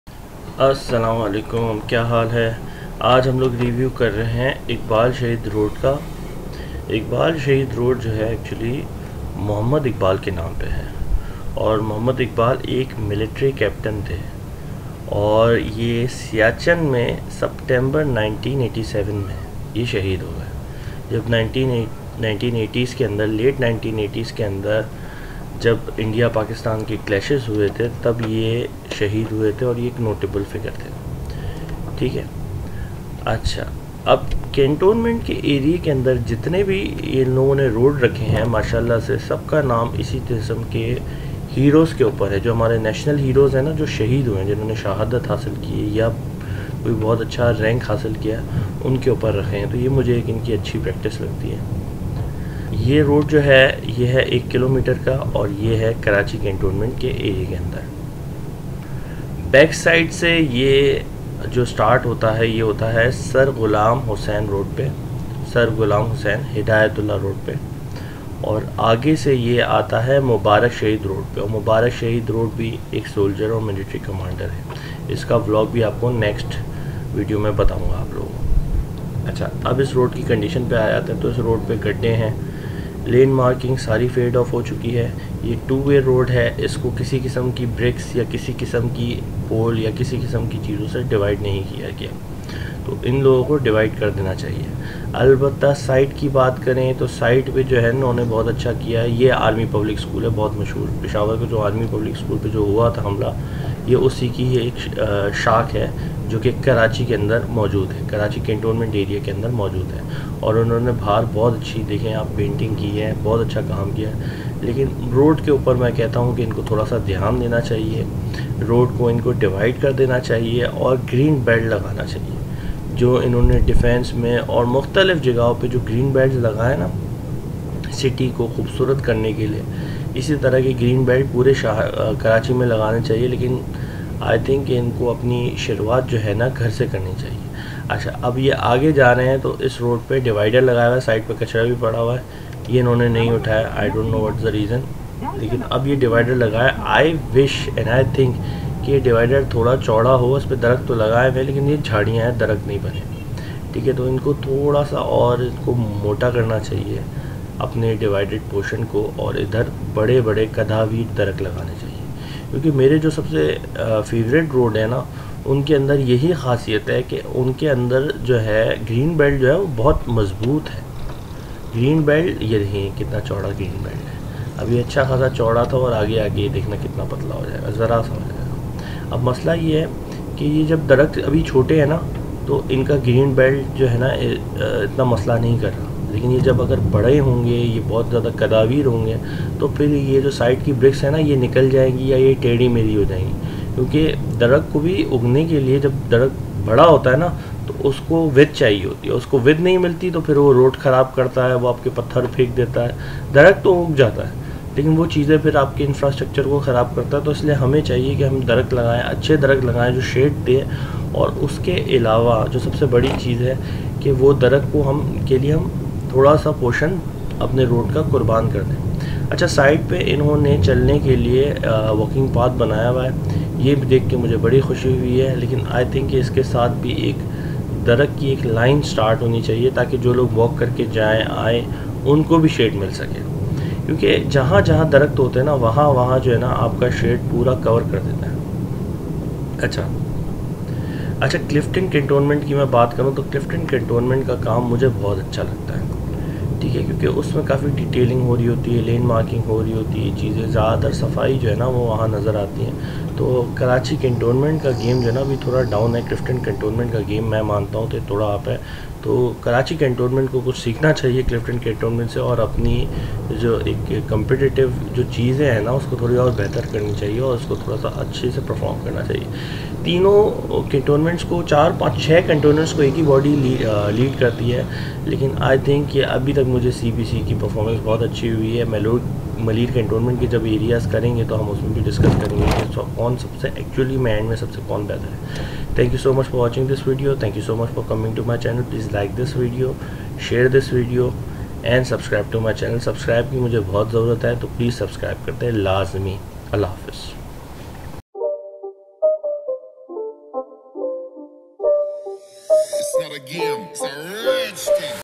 Iqbal Shaheed Road | Karachi Cantonment | Karachi Street View | 29th June 2023
Reviewing iqbal shaheed road from Sir Ghulam hussain hidayatullah road to Mubarak Shaheed road in Karachi Cantonment Area